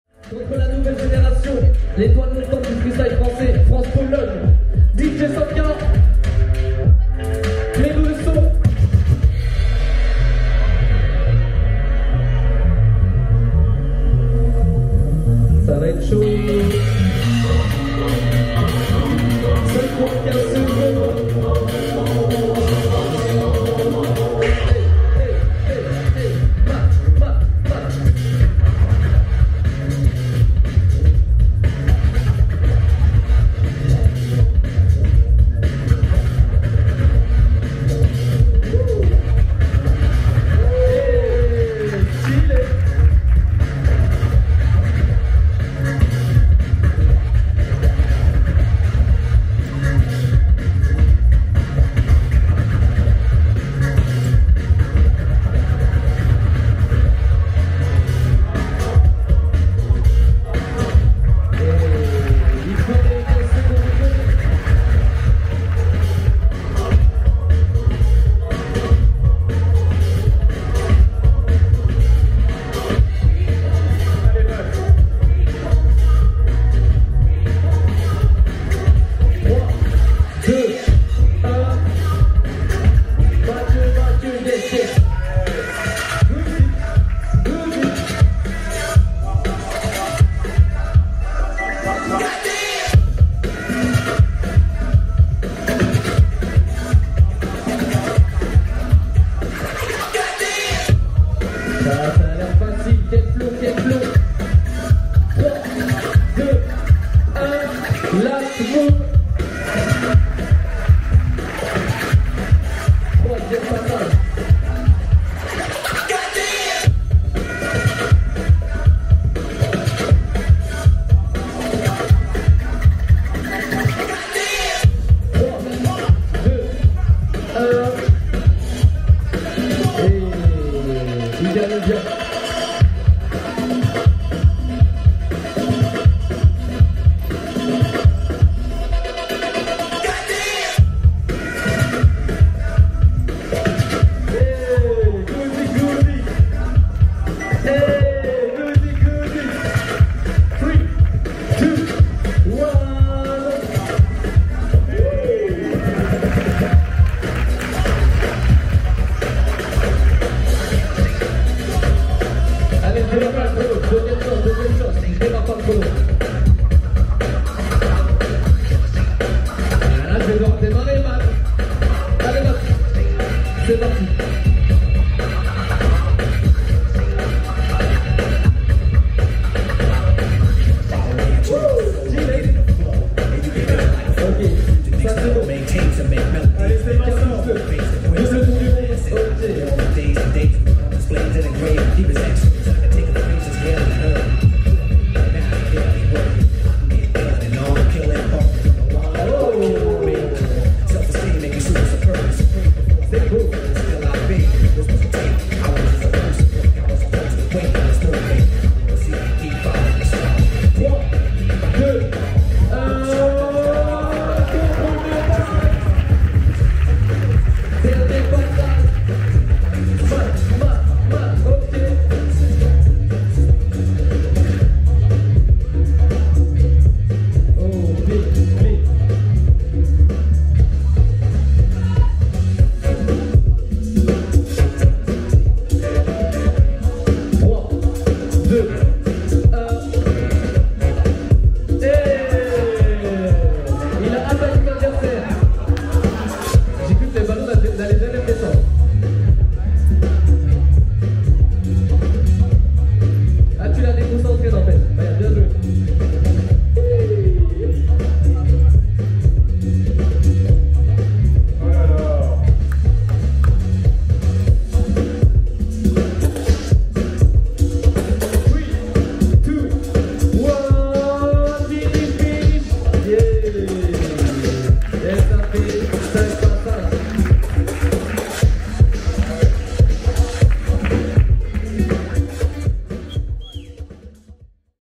Guzik vs Mat - Top 16 | Footstyle Cup 2022
🏆 | Winner: Guzik